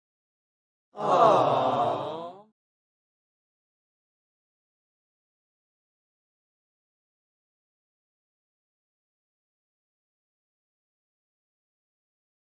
Crowd Aww - Sound Effect
Crowd reacting to cuteness sound effect :)